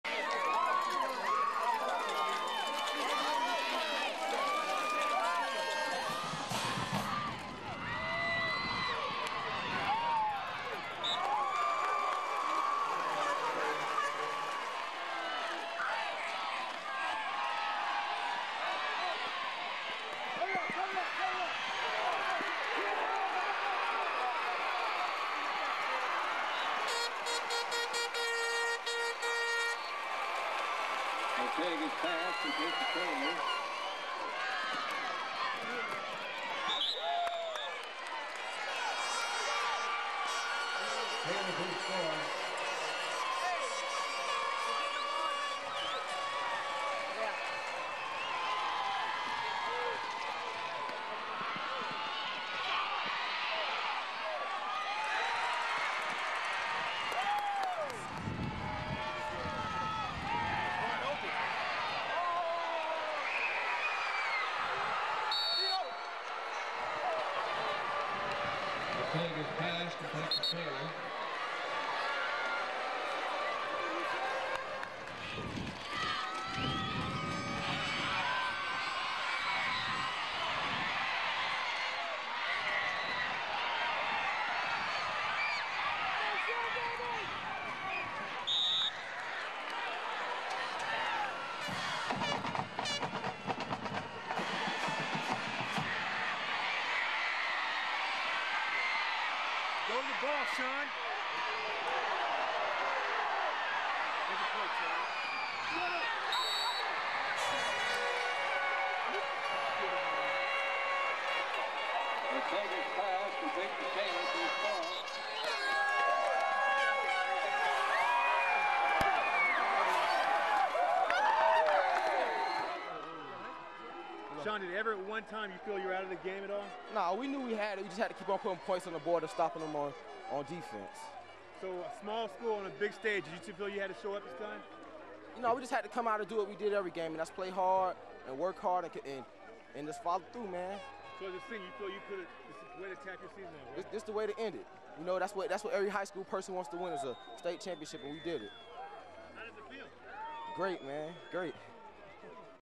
Sean Taylor State Championship 2000 Gulliver
Raw highlights of Sean Taylor from the 2000 Gulliver State Championship team.  Must contact NBC6 for permission to use.